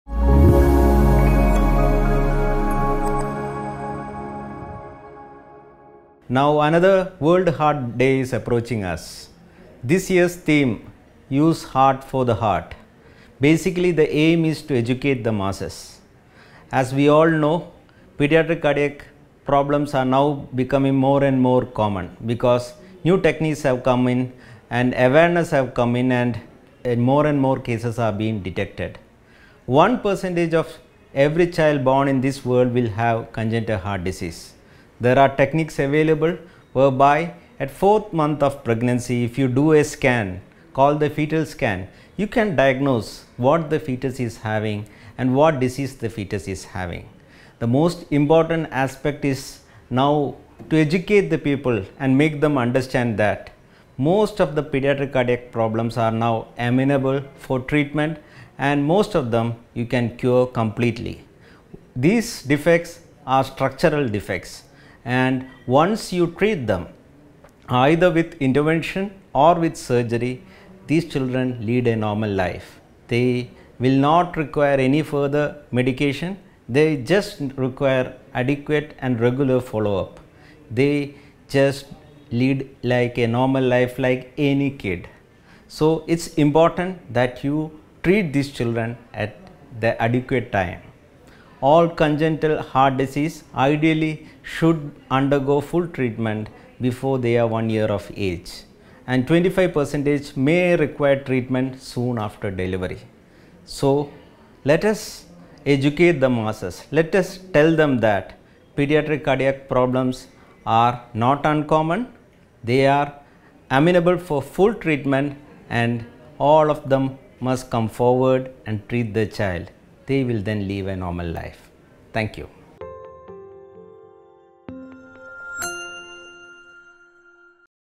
Hridayamrutham | World Heart Day 2023 | Congenital Heart Diseases Insights | Amrita Hospitals
Join us for World Heart Day 2023 as we delve into the theme "Use Heart for Every Heart" with Dr. Brijesh PK, Clinical Associate Professor at the Centre for Aortic Diseases and Marfan Syndrome, Amrita Valve Repair Centre. Dr. PK, an expert in Pediatric and Congenital Heart Surgery, as well as Pediatric Heart Transplantation at Amrita Hospitals, sheds light on the importance of congenital heart diseases, pediatric cardiology, and holistic care. He highlights that 1% of children worldwide are born with congenital heart diseases, and through advanced technologies, we can diagnose these issues during fetal scans and achieve complete cardiac issue cures. Listen to his expert insights.